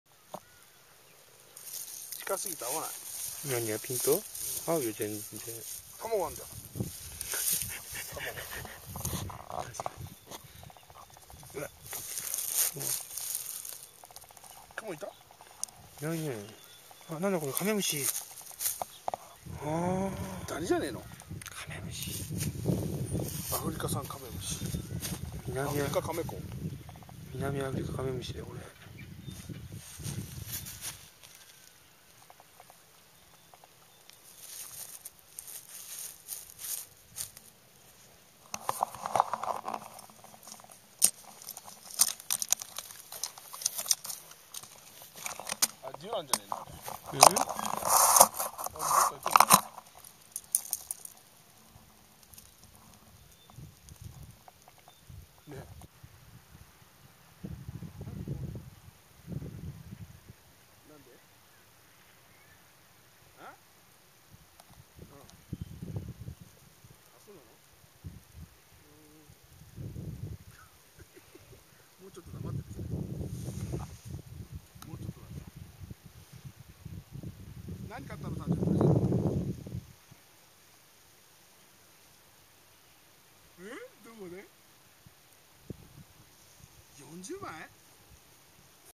Insect Watching - South Africa No.2 : True bug
I found a small ture bug in Bloemfontein, South Africa in 18th July 2008. I don't know its scientific name.